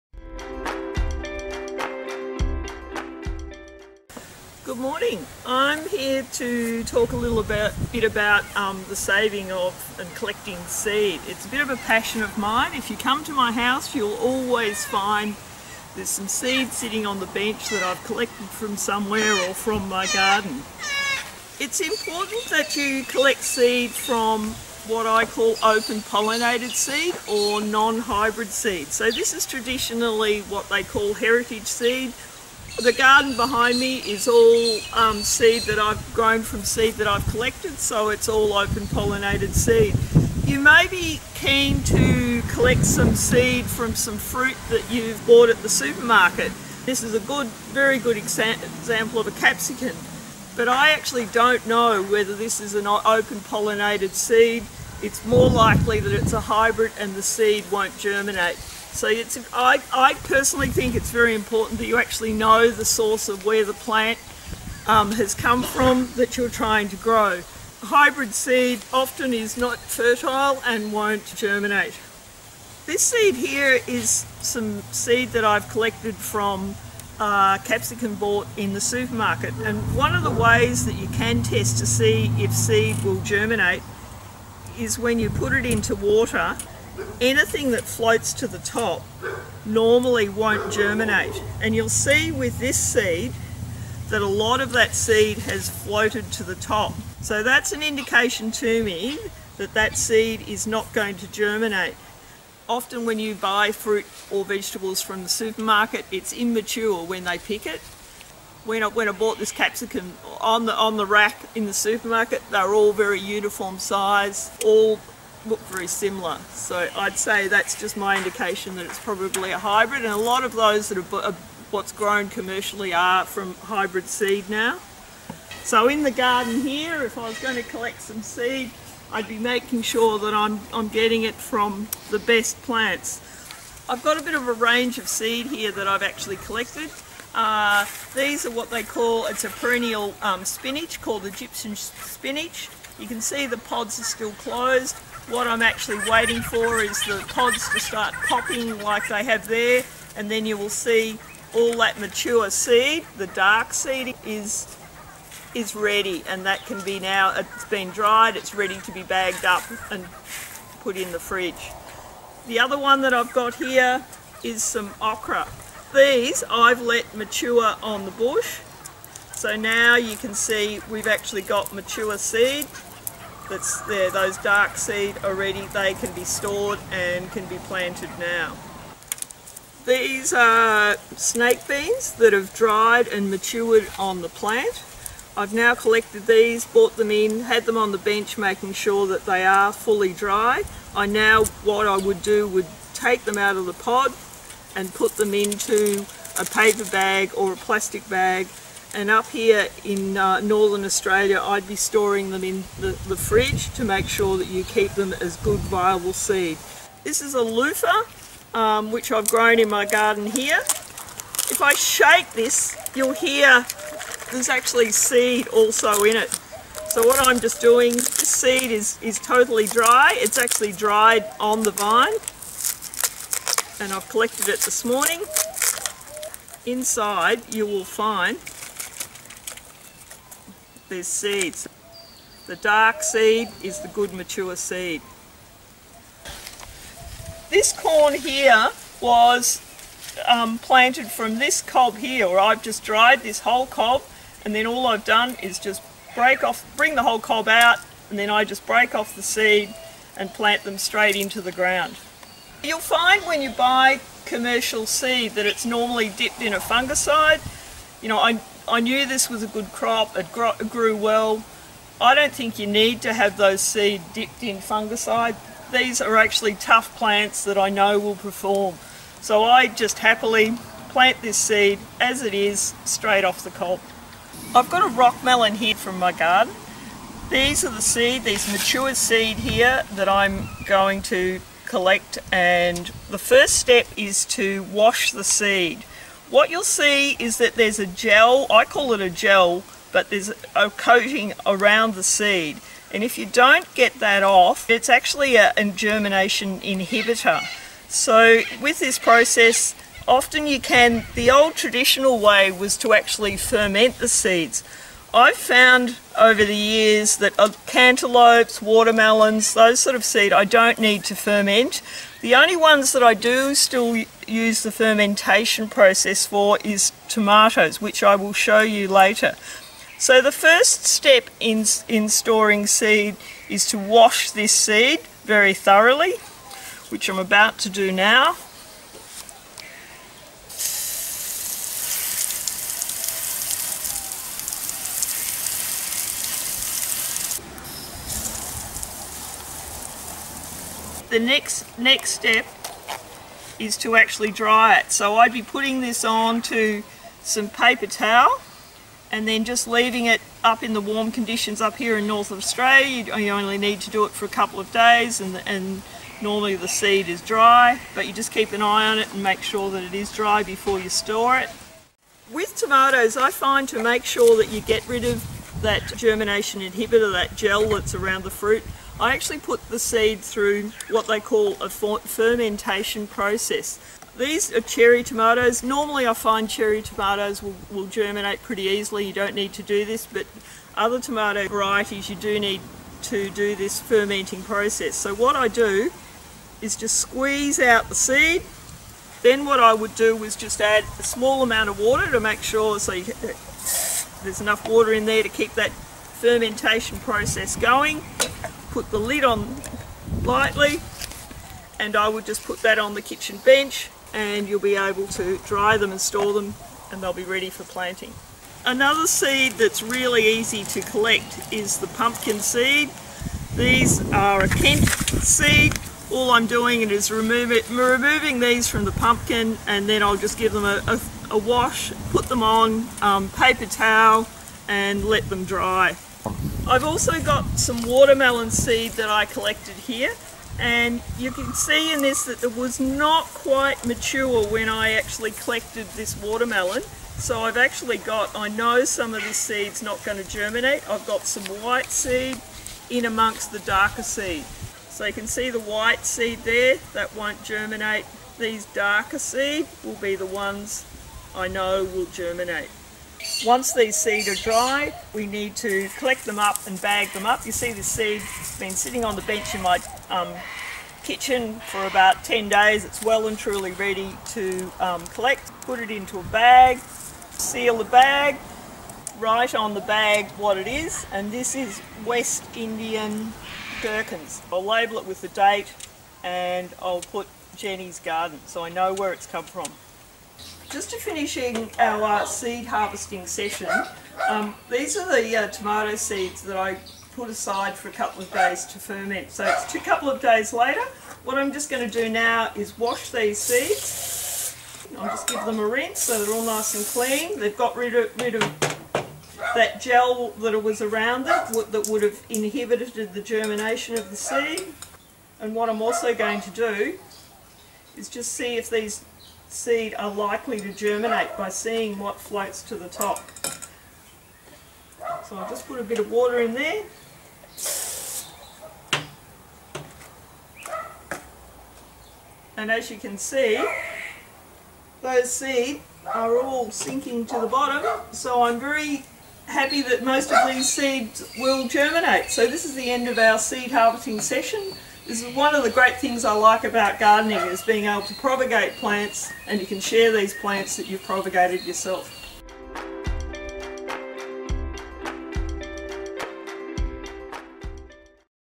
EON - Harvesting Seeds
A Training Clip produced by the EON Foundation. EON Foundation builds edible gardens in remote Aboriginal schools and communities, providing education and training in nutrition and horticulture. Visit eon.org.au.